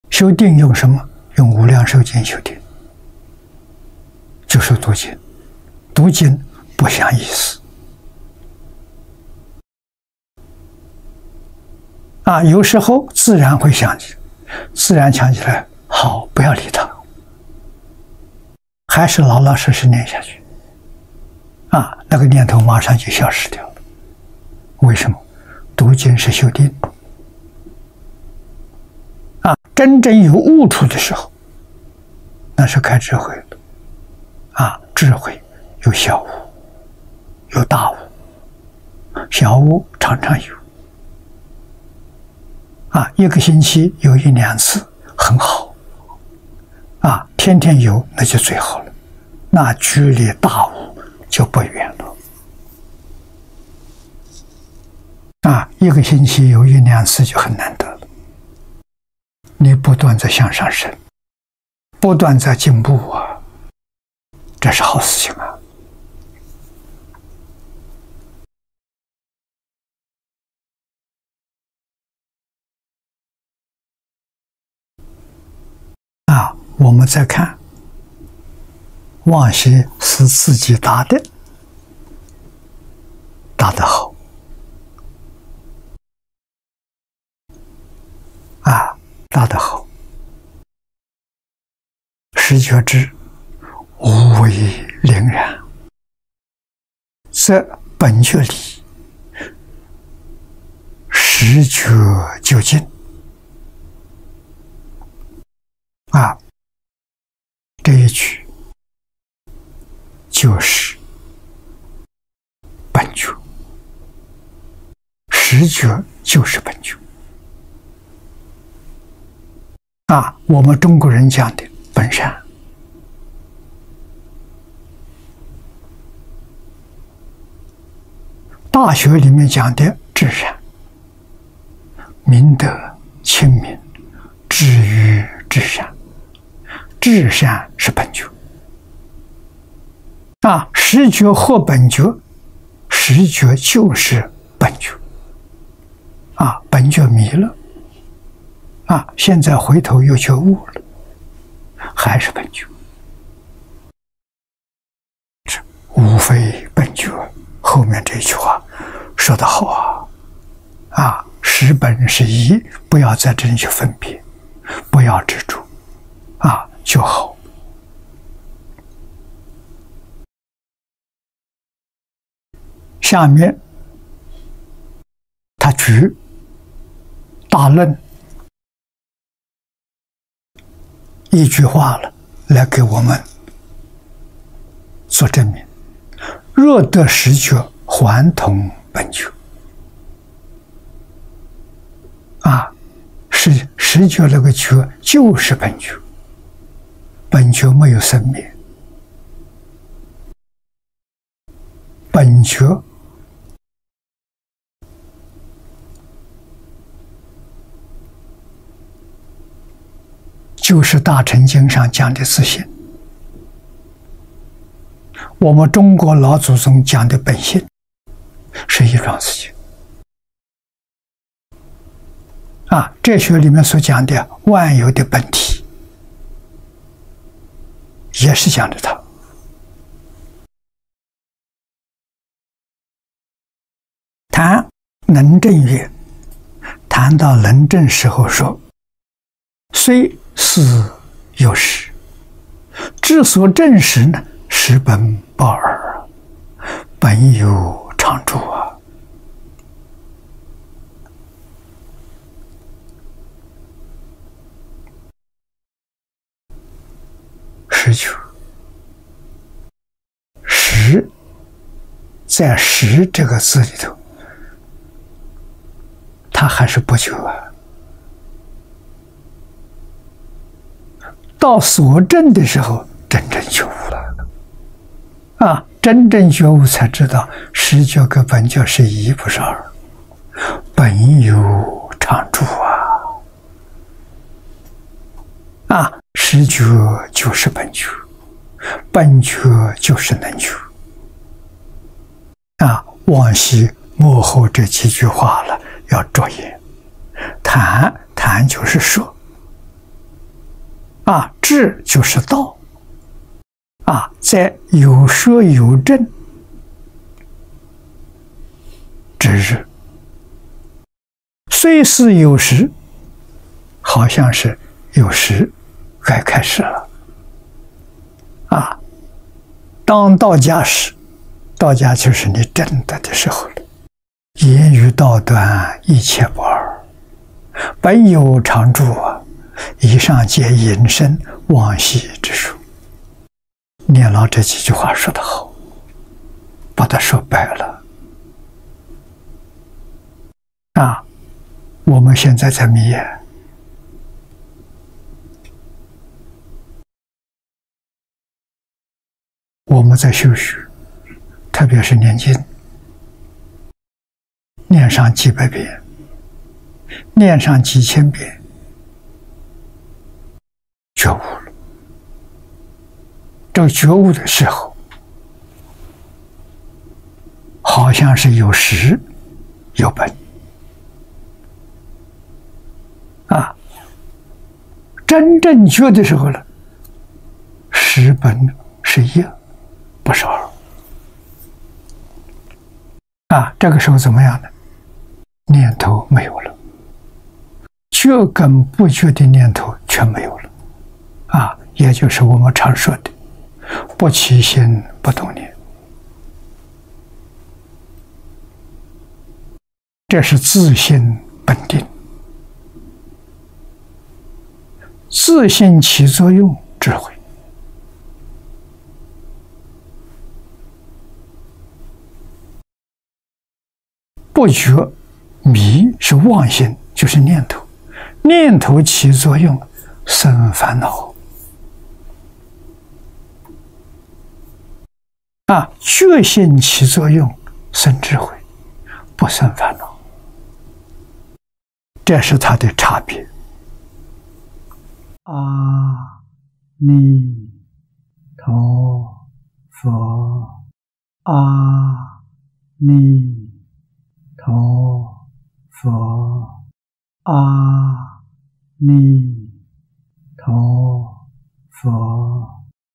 🙏淨空老法師🙏當到家的時候 會怎樣呢?🙏
修定用什麼？用《無量壽經》修定，就是讀經。讀經不想意思，有時候自然會想起，自然想起來好，不要理它，還是老老實實念下去，那個念頭馬上就消失掉了。為什麼？讀經是修定。真正有悟處的時候，那是開智慧了。智慧有小悟、有大悟。小悟常常有，一個星期有一、二次很好，天天有那就最好了，那距離大悟就不遠了。一個星期有一、二次就很難得，你不斷在向上升，不斷在進步，這是好事情。
　　我們再看望西師自己答的，答得好。始覺智，無為凝然，則本覺理，始覺究竟。這一句就是本覺，始覺就是本覺，我們中國人講的本善。《大學》裡面講的至善、明德、親民、止於至善，至善是本覺。始覺合本覺，始覺就是本覺，本覺迷了，現在回頭又覺悟了，還是本覺。無非本覺，後面這句話說得好，始本是一，不要在這裡去分別，不要執著就好。下面他舉《釋大衍論》一句話來給我們做證明，「若得始覺，還同本覺」。始覺那個覺就是本覺，本覺沒有生滅，本覺就是大乘經上講的自性，我們中國老祖宗講的本性，是一樁事情，哲學裡面所講的萬有的本體，也是講的它。「談能證曰」，談到能證時候說：「雖似有始，至所證時，始本不二，本有常住」。始覺，始，在始這個字裡頭，他還是不覺，到所證的時候真正覺悟，真正覺悟才知道，始覺跟本覺是一不是二，本有常住。始覺就是本覺，本覺就是能覺。
　　望西末後這幾句話要著眼。談，談就是說，至就是到。「在有說有證之日，雖似有始」，好像是有始，開始了。「當到家時」，到家就是你證得的時候，「言語道斷，一切不二，本有常住。以上皆引申望西之說」。念老這幾句話說得好，把它說白了。我們現在在迷，我們在修學，特別是念經，念上幾百遍、念上幾千遍，覺悟了，這個覺悟的時候好像是有始有本，真正覺的時候，始本是一不是二。這個時候怎麼樣？念頭沒有了，覺跟不覺的念頭全沒有了，也就是我們常說的，不起心不動念，這是自性本定，自性起作用，智慧。不覺，迷，是妄心，就是念頭，念頭起作用生煩惱；覺心起作用生智慧，不生煩惱，這是它的差別。